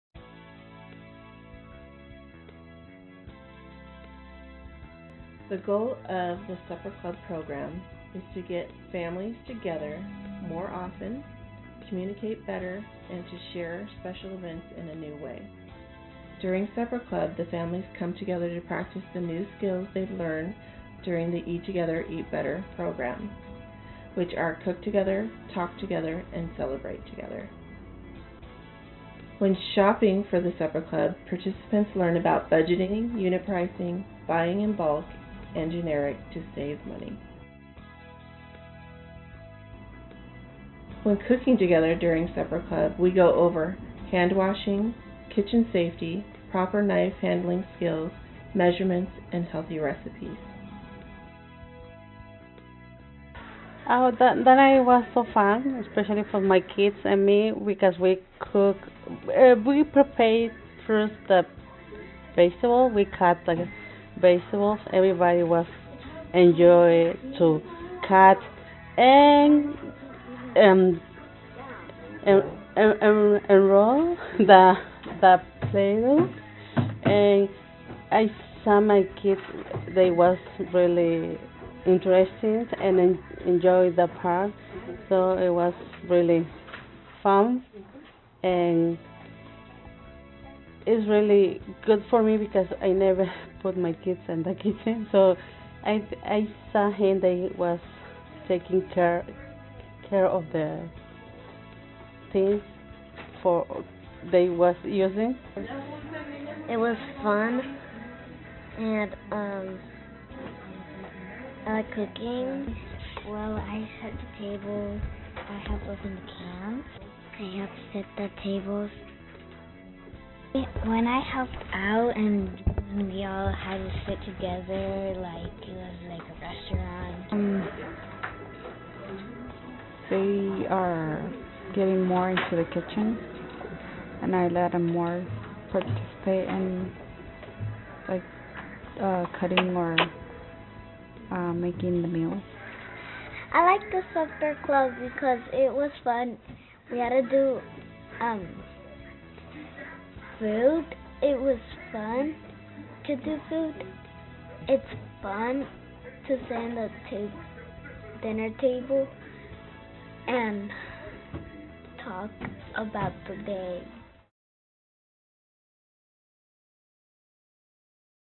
Digital Evaluation of Supper Club
This is a digital review I created at Washington State University Clark County Extension, during my internship. I feel that I was able to show how my Human Development Degree is enhanced by my formal minor in Digital Technology and Culture.